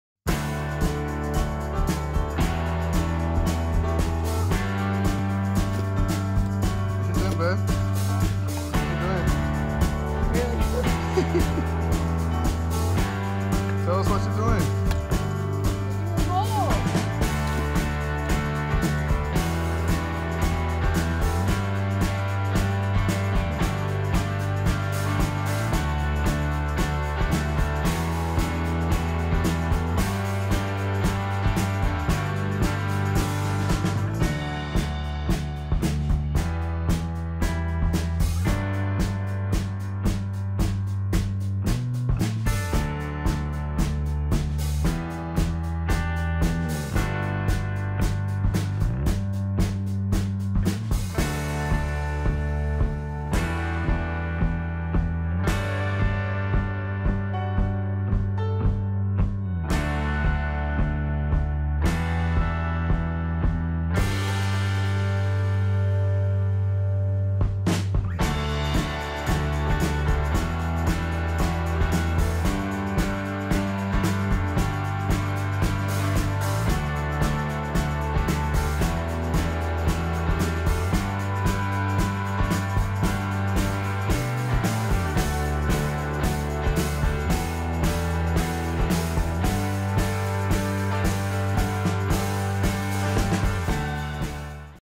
Building Sheds around here
Building a shed it's a great opportunity to spend time with friends and family and learn something new.